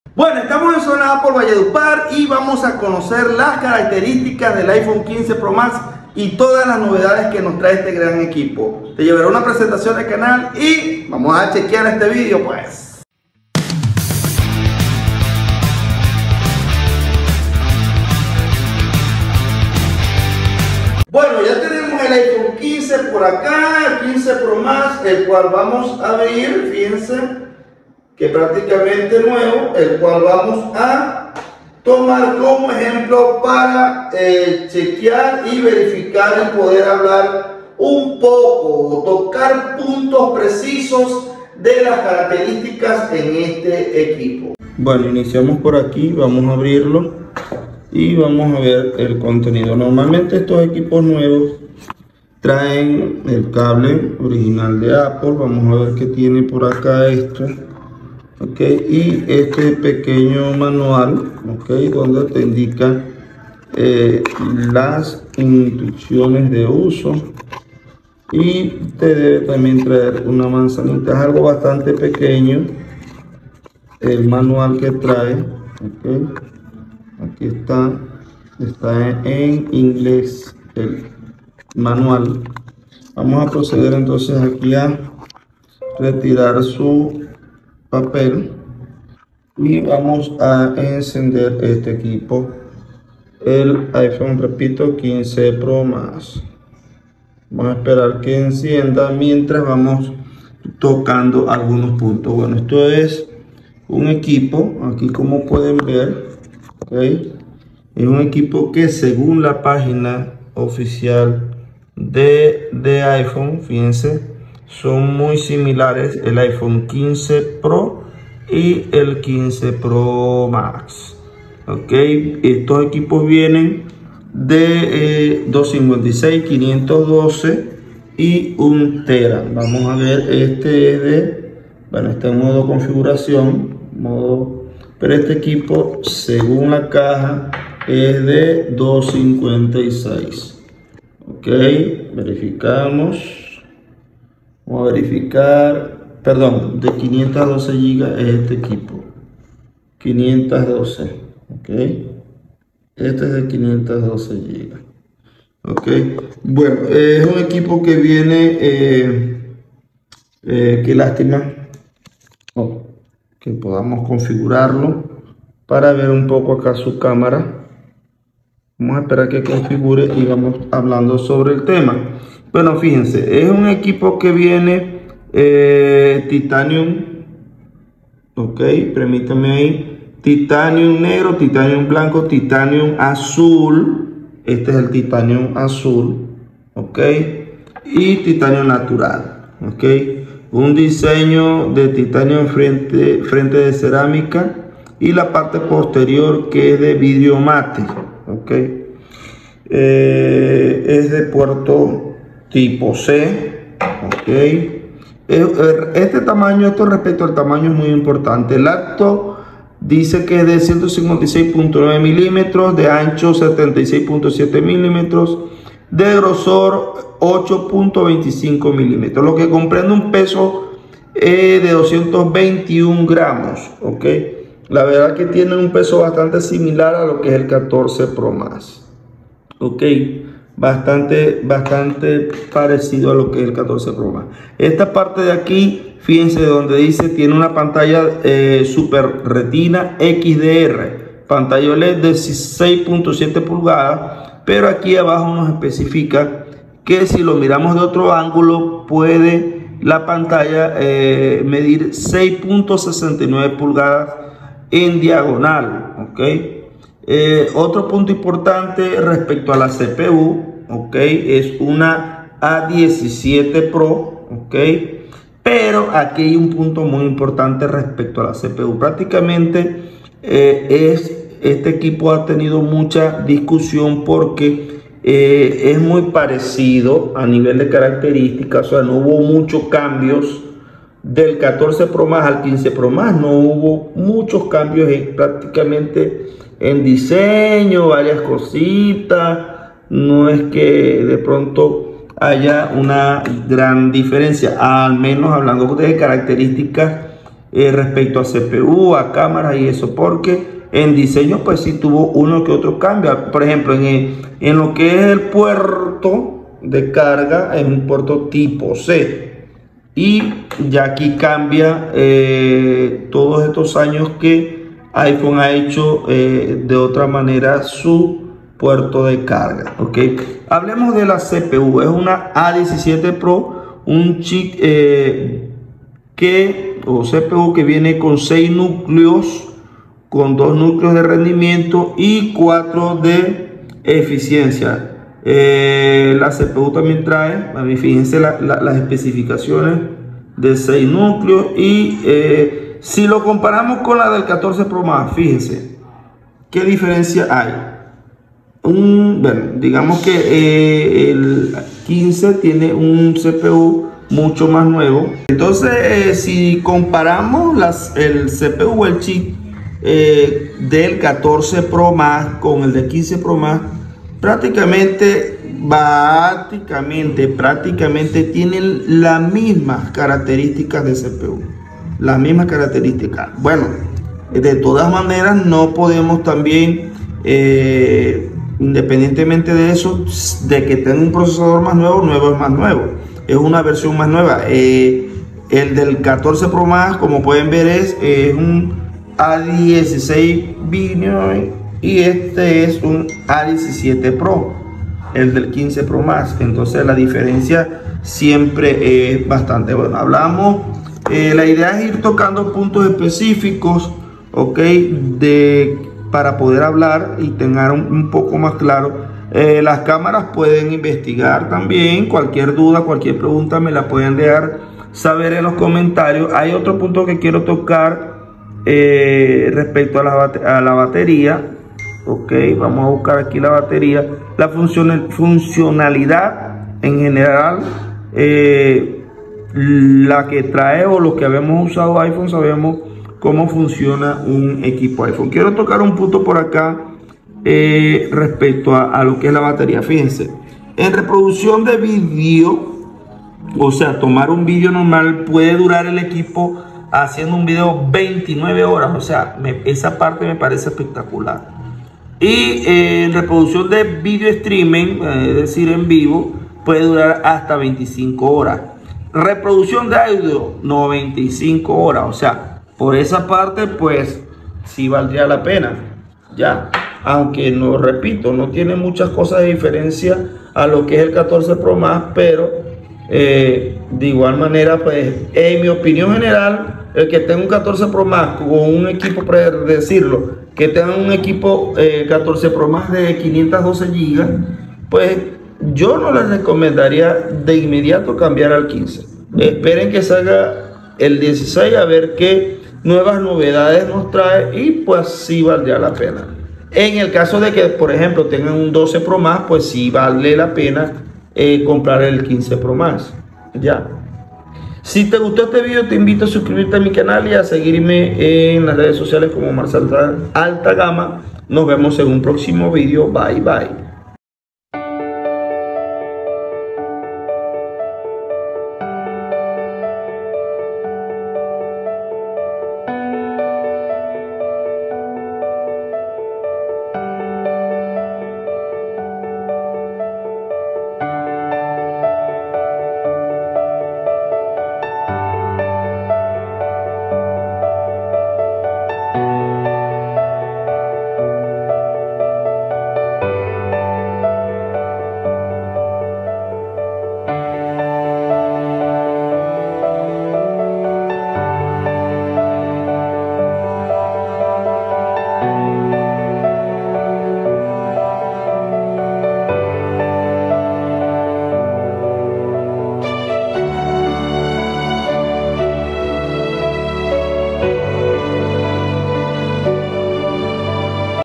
iPhone 15 pro max características y novedades 🍎😍👈
apple iphone iphone15promax valleduparcolombia características y novedades de iPhone 15 pro max 😍 desde la tienda zona apple en VALLEDUPAR